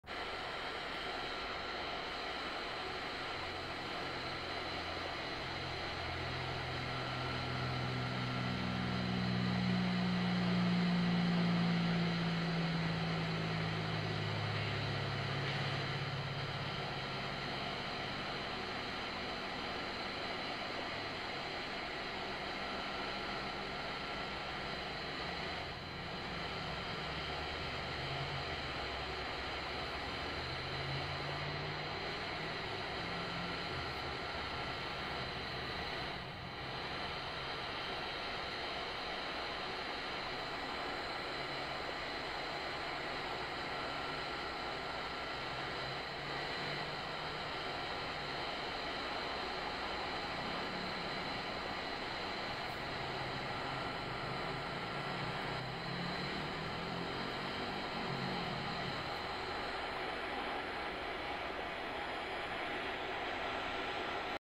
the unexplainable
this amazing footage was taken in a hallway and I can't explain it. any ideas.